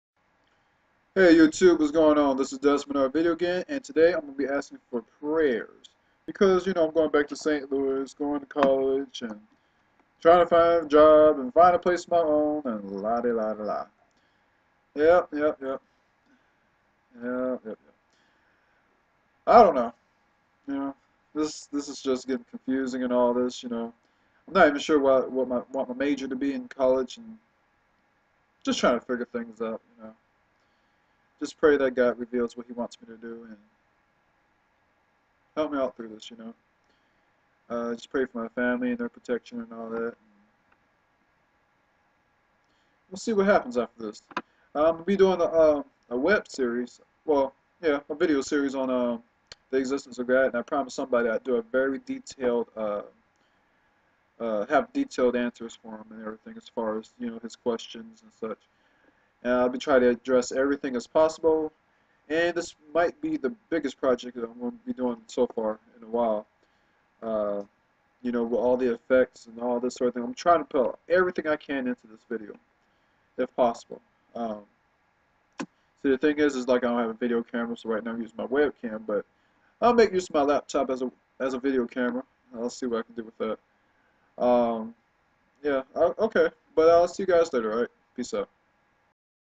Asking For Prayer Jan 19th 2010
Just asking for prayer, I am under a lot fo stress trying to get my life together now, a new job, new place, school, etc lol I just need God right now :)